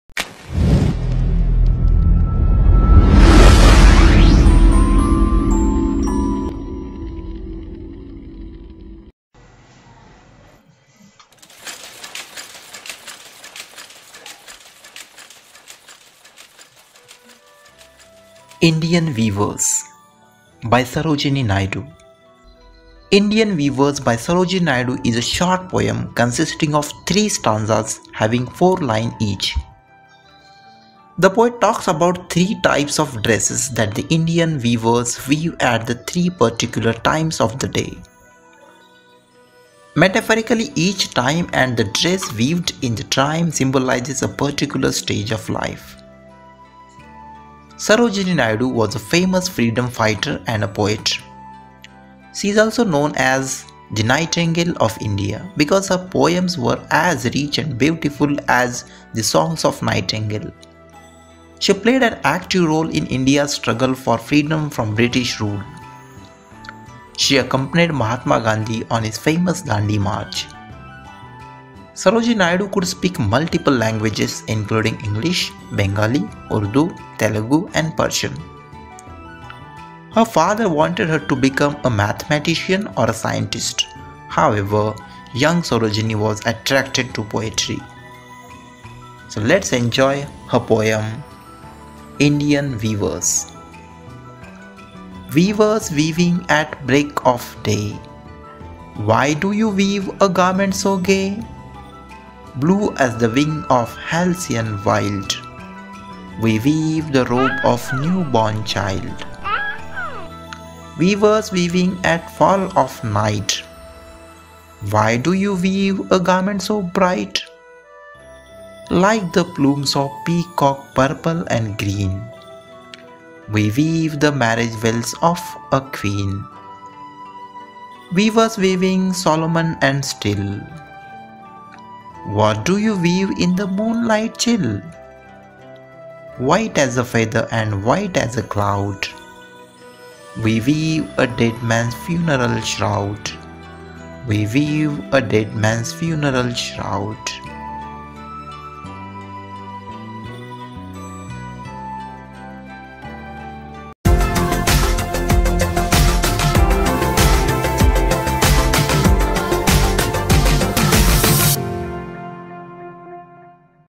Indian weavers| Sarojini Naidu|English poem
The ‘Indian Weavers’ is a small lyric in three stanzas of four lines each included in the “Folk Songs” section of The Golden Threshold. In each stanza, a question is asked in the first two lines, and it is answered in the remaining two lines. Weaving is one of the most important of the folk vocations in India, and the weavers are important folk characters. Sarojini Naidu has skillfully retained the simplicity and lilting music of a folk song, even though the lyric has a symbolic significance and a rich texture having layers within layers within of meaning.